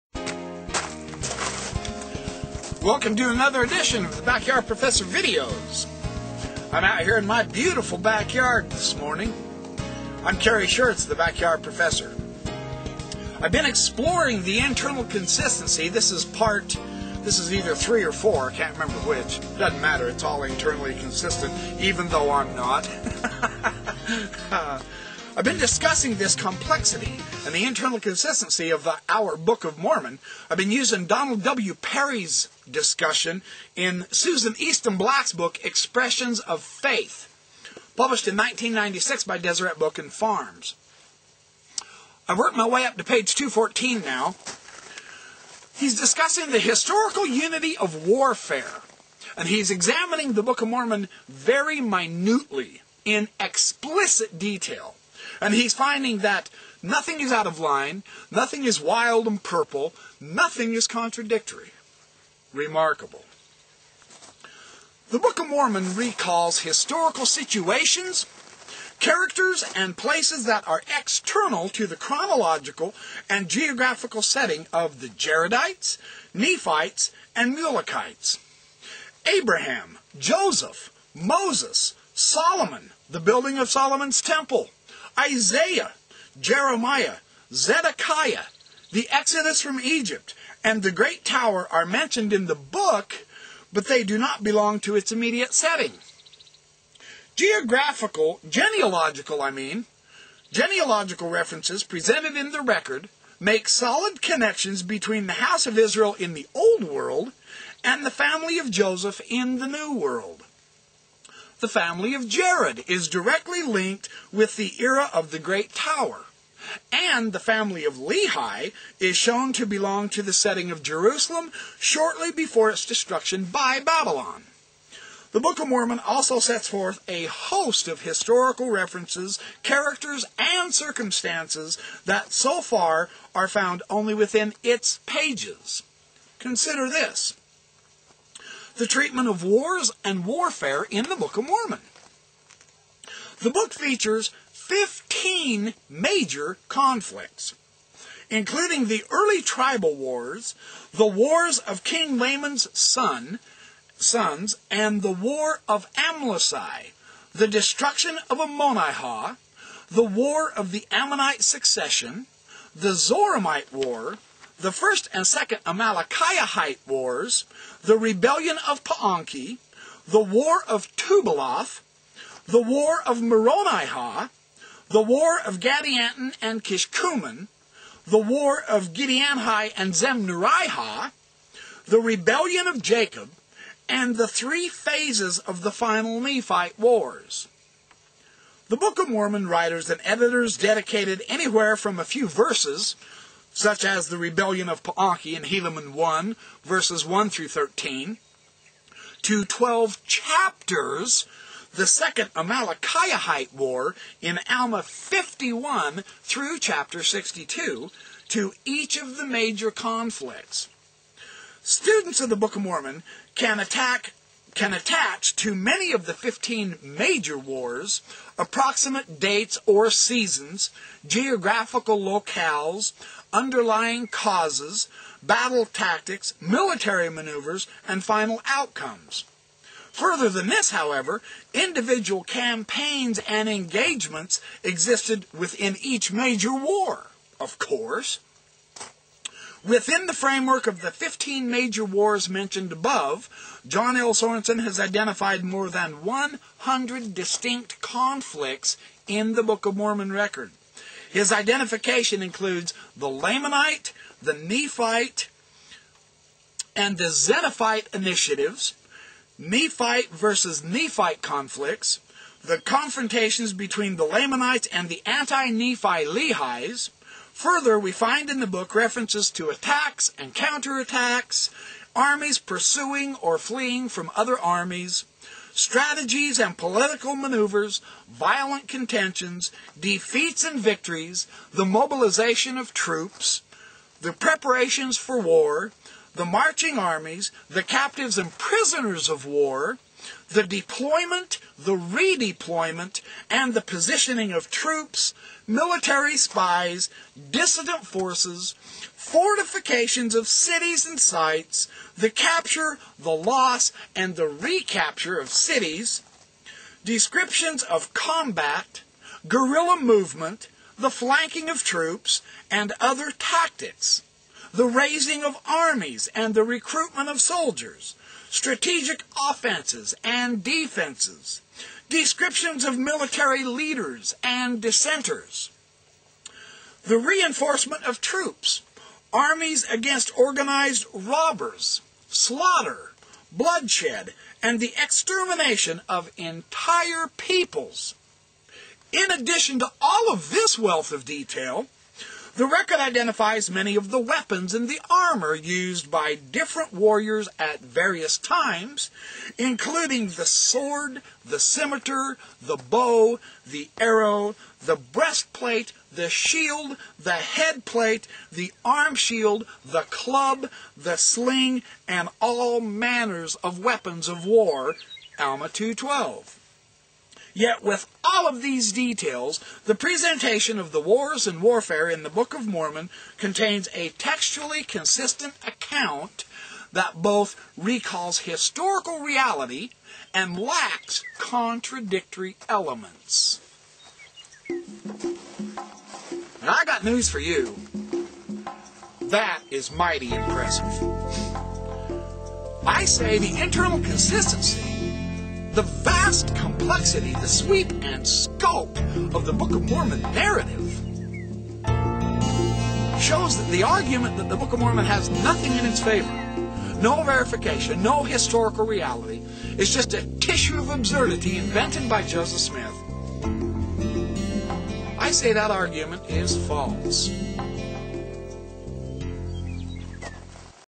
Realistic Book of Mormon Evidences
Part 4 of the Internal Consistency of the Book of Mormon discussing the intricacies of warfare and all its tactics.